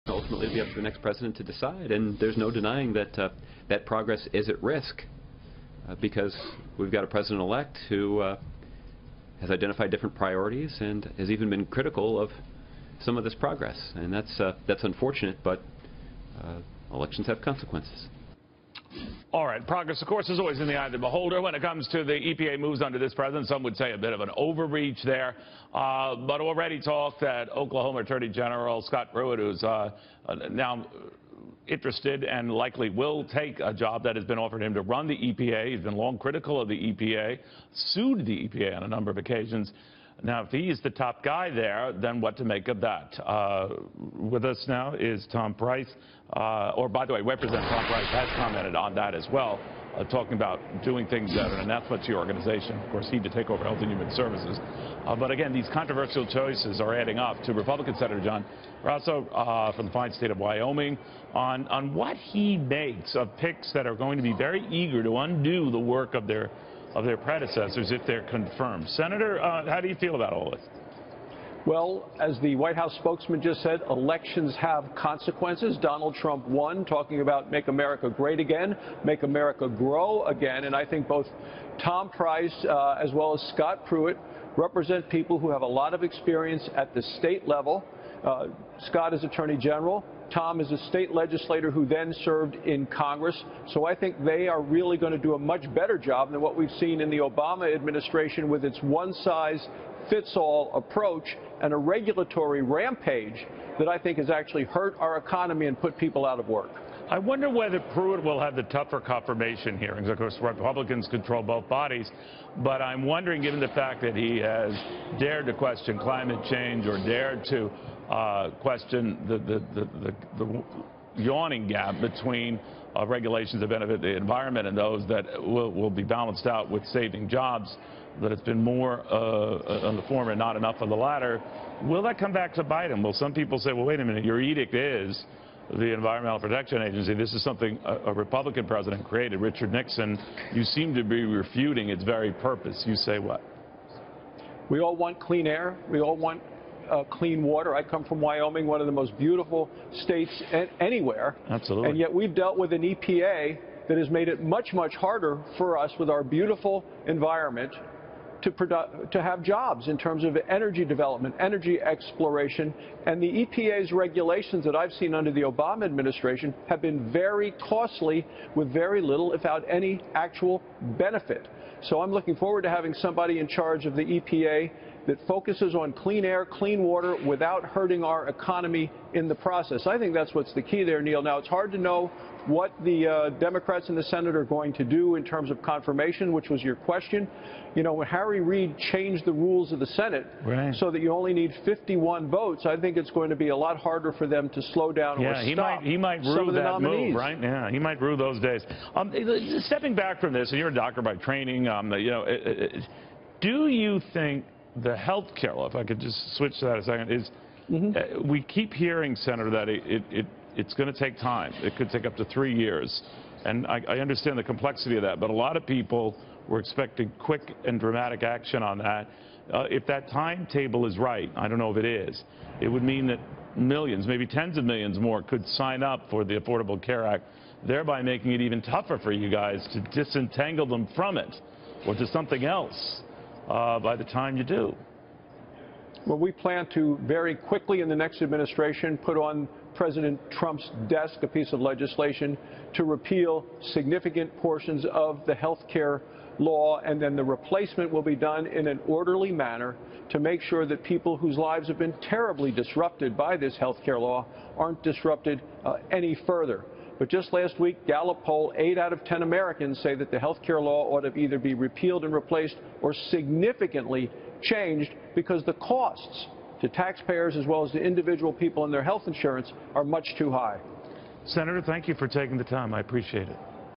Sen. Barrasso: EPA has been costly
Senator John Barrasso (R-WY) on Donald Trump’s pick for Scott Pruitt as the head of the EPA.